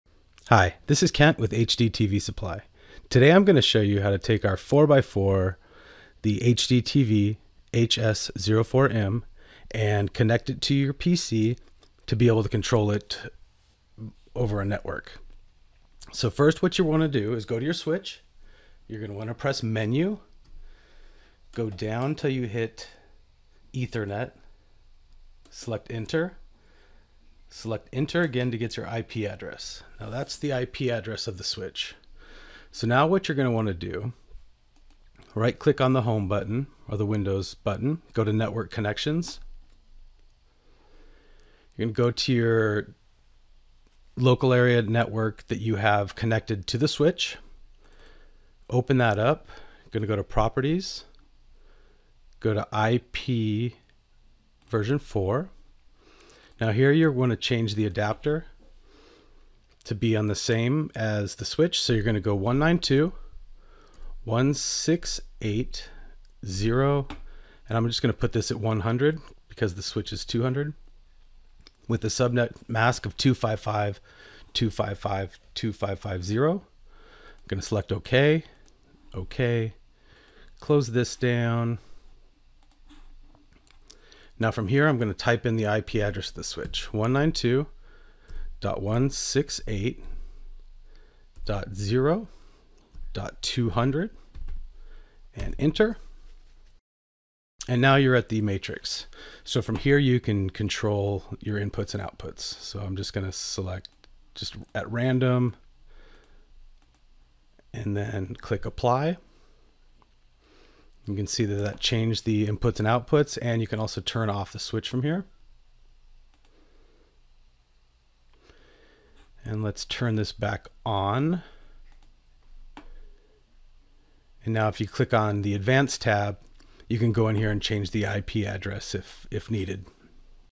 WolfPack™ 4x4 HDMI Switch with Android App
Controlling WolfPack™ 4x4 HDMI Switch by Web GUI